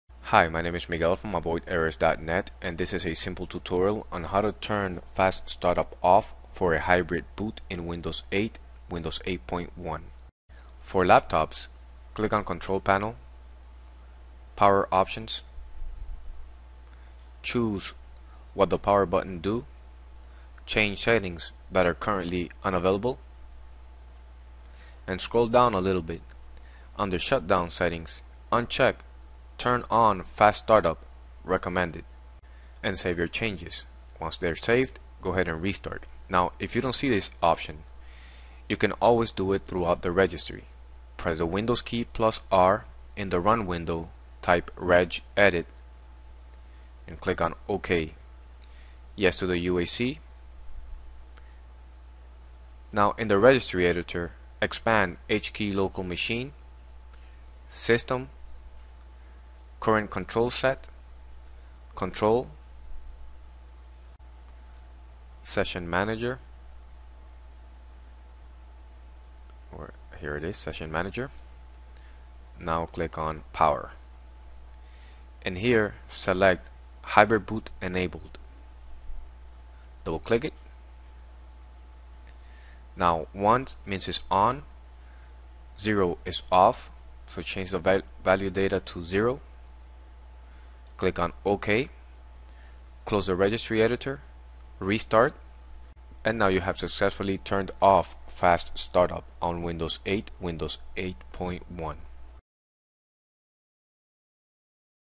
How to Turn Fast Startup Off for a Hybrid or Dual Boot in Windows 8 and 8.1
Cant see the grub menu while dual booting windows 8. Windows 8.x has a new feature called Fast Startup (hybrid boot or hybrid Shutdown) to help your PC Boot faster